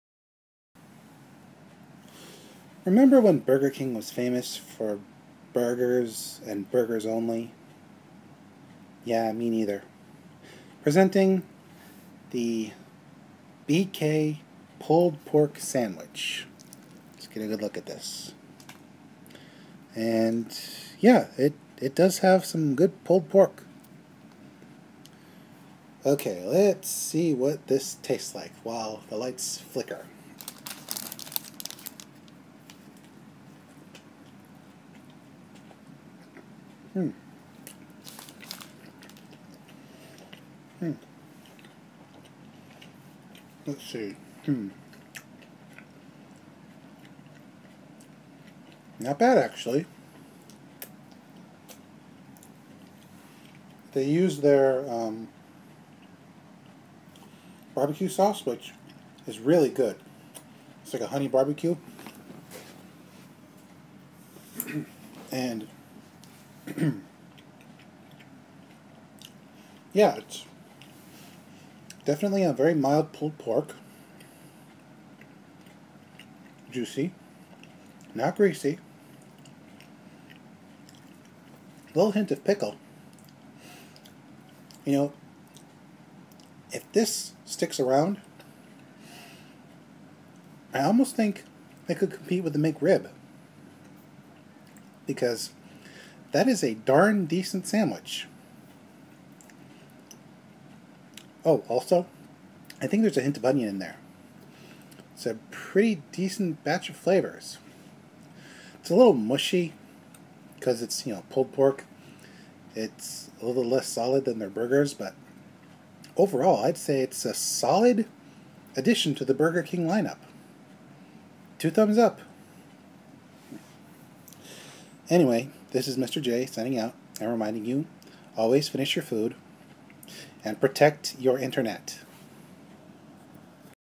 Mr J Tries the BK Pulled Pork Sandwich
Once again, we have a novelty sandwich from Burger King, but this time, it has some staying power! If you like pulled pork, this might be a good go-to if your wallet is a bit light.